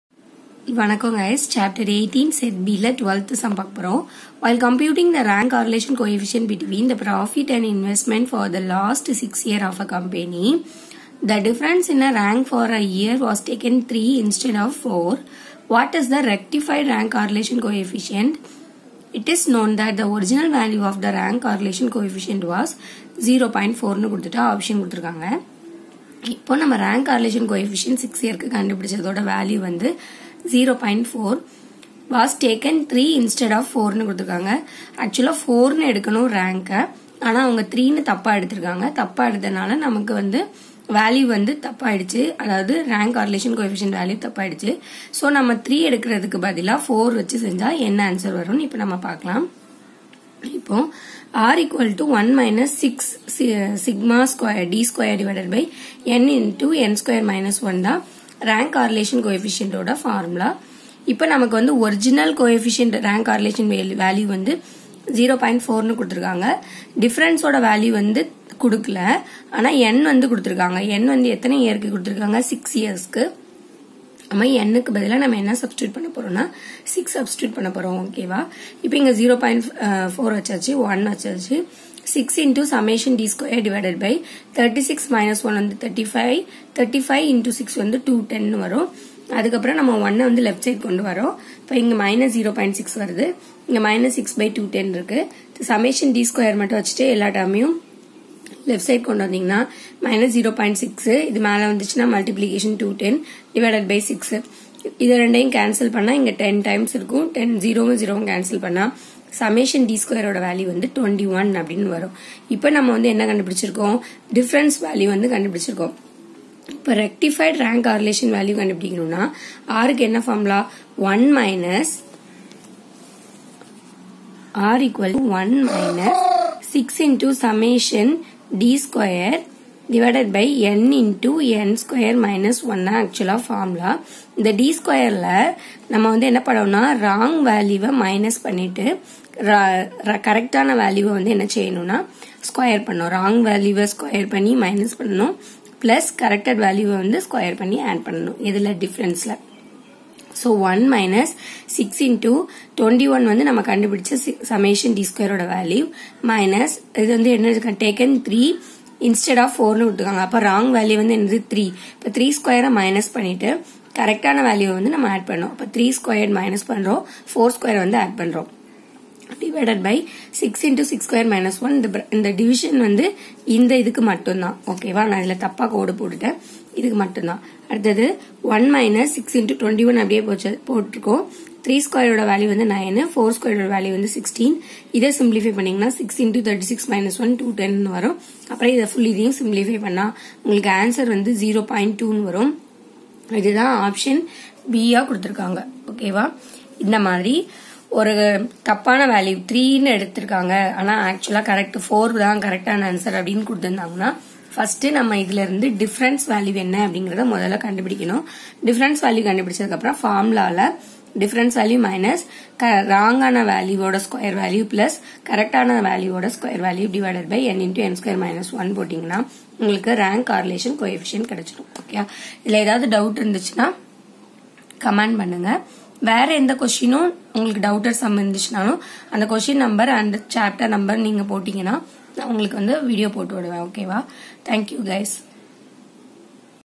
CA Foundation Maths In Tamil|ch18,Set B,Q12|While computing rank correlation between profit and ..
CA Foundation Maths Statistics Chapter 18 Set B, 12 th question
12)While computing rank correlation between profit and investment for the last 6years of company the difference in rank for a year was taken 3 instead of 4.What is the number of students in the group?